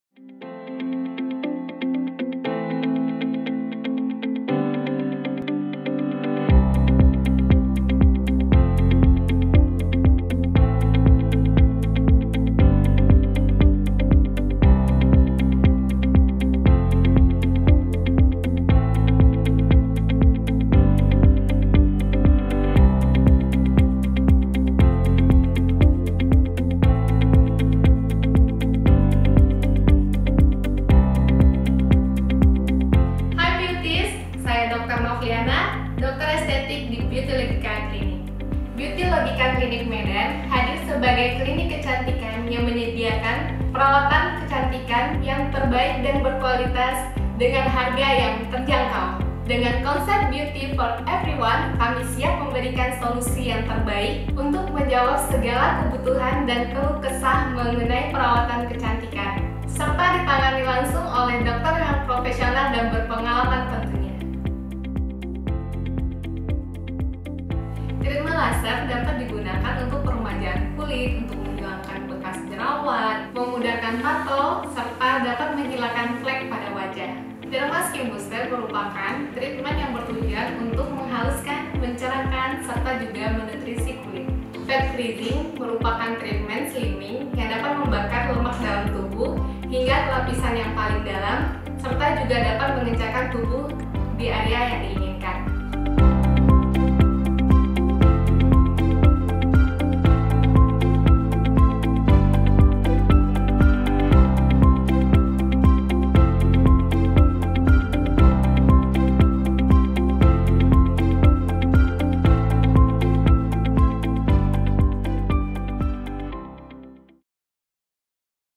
BEAUTY FOR EVERYONE; BEAUTYLOGICA CLINIC MEDAN
Hi Beauties, siapa yang sudah tahu bahwa Beautylogica Clinic hadir di Medan ??

Sebagai salah satu klinik kecantikan, kami akan selalu berusaha memberikan  pelayanan yang terbaik untuk Beauties dimanapun kami berada.

Temukan treatment-treatment terbaik kami di Beautylogica Clinic sekarang juga.
Informasi lebih lanjut, hubungi Beautylogica Clinic di:
🔎 Plastic Surgery, Aesthetic Treatment, Hair Care
📍  Pejaten, Sudirman, Solo, Medan